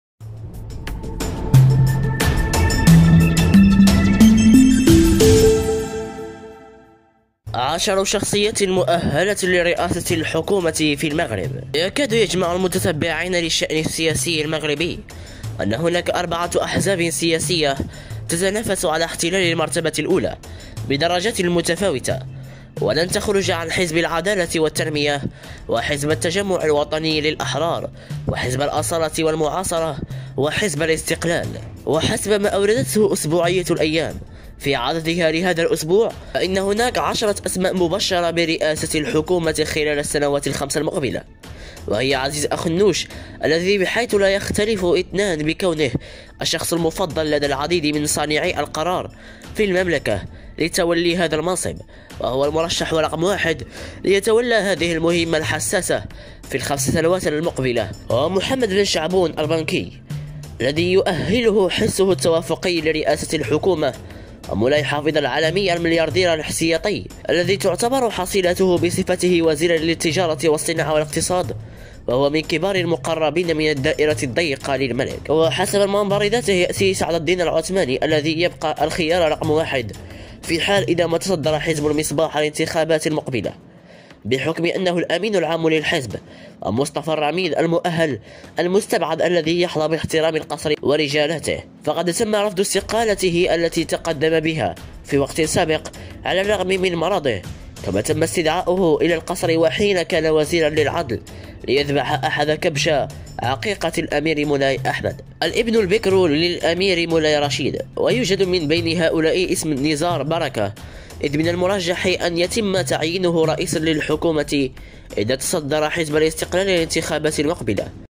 10 شخصيات مؤهلة لرئاسة الحكومة في المغرب التي سيختار منهم الملك رئيس الحكومة الجديدة
هؤلاء هم الشخصيات العشر التي سيختار منهم
الملك رئيس الحكومة الجديدة

10 شخصيات مؤهلة لرئاسة الحكومة في المغرب
يكاد يجمع المتتبعين للشأن السياسي المغربي، أن هناك
أربعة أحزاب سياسية تتنافس على احتلال المرتبة الأولى
بدرجات متفاوتة، ولن تخرج عن حزب العدالة والتنمية
ووحزب التجمع الوطني للأحرار وحزب الأصالة
والمعاصرة وحزب الاستقلال.